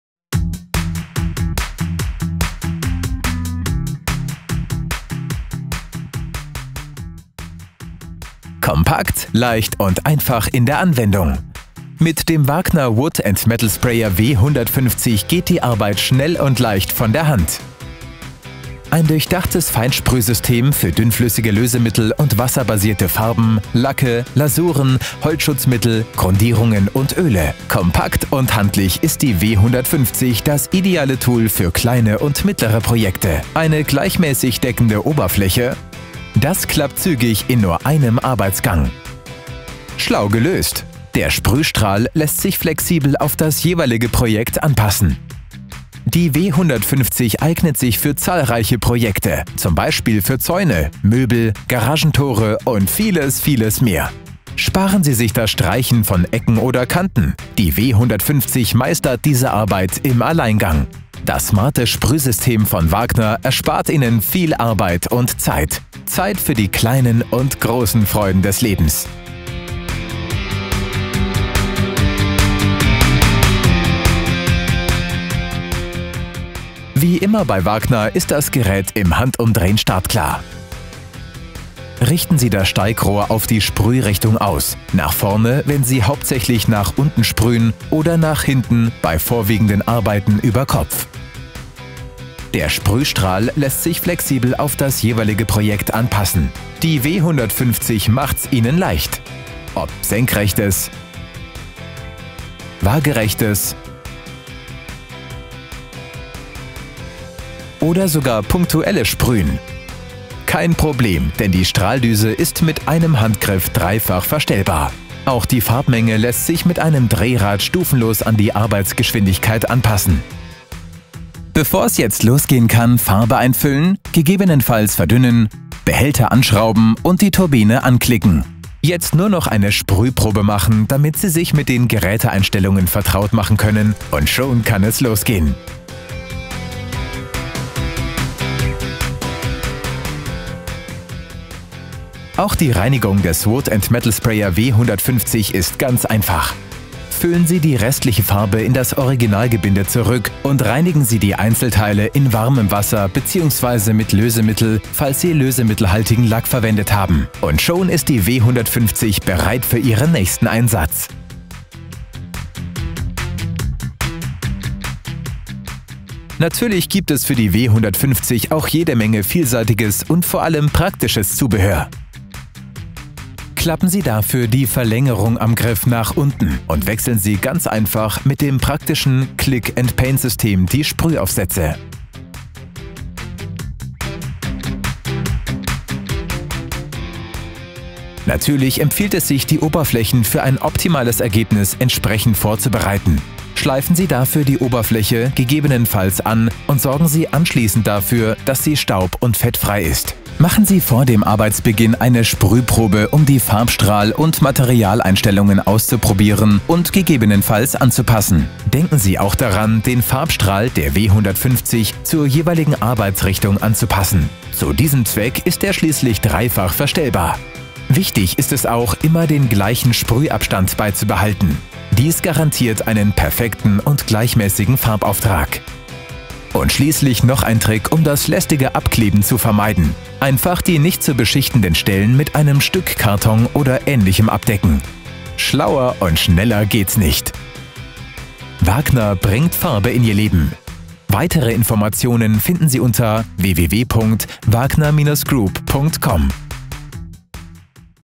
Wood&Metal Sprayer W 150 von WAGNER - Produktanleitung
Besonders für Projekte rund ums Haus eignet sich das Lack und Lasuren Sprühsystem W 150 hervorragend. Vom Komplettanstrich Ihres Gartenhauses oder dem Holzzaun bis hin zur Pflege oder Verschönerung der Sitzmöbeln - W 150 ist der optimale Begleiter bei all Ihren Vorhaben.

0:00 Anwendungsbeispiele
1:11 Inbetriebnahme
2:18 Reinigung
2:44 Zubehör
3:03 Tipps & Tricks

- Bitte beachtet zusätzlich die Sicherheitshinweise in der Betriebsanleitung -